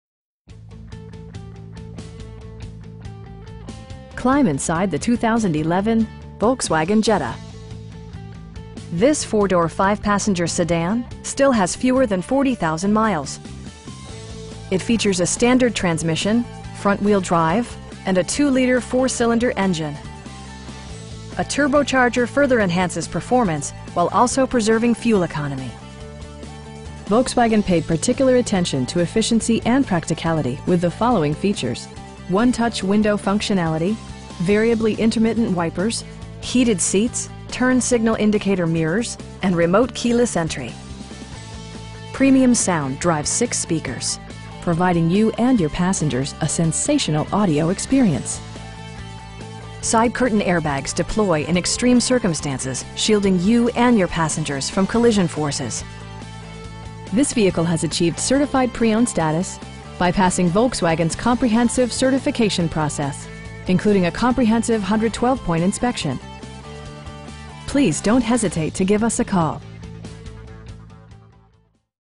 2011 Volkswagen Jetta TDI LE in Alpharetta, GA 30009
1550 Mansell Road
in Alpharetta, GA 30009

Climb inside the 2011 Volkswagen Jetta. This 4 door, 5 passenger sedan still has fewer than 40,000 miles! It features a standard transmission, front-wheel drive, and a 2 liter 4 cylinder engine. A turbocharger further enhances performance, while also preserving fuel economy. Volkswagen paid particular attention to efficiency and practicality with the following features: 1-touch window functionality, variably intermittent wipers, heated seats, turn signal indicator mirrors, power windows, cruise control, and remote keyless entry. Premium sound drives 6 speakers, providing you and your passengers a sensational audio experience. Side curtain airbags deploy in extreme circumstances, shielding you and your passengers from collision forces. This vehicle has achieved Certified Pre-Owned status, by passing Volkswagen's comprehensive certification process, including a comprehensive 112 point inspection! We pride ourselves in consistently exceeding our customer's expectations. Please don't hesitate to give us a call.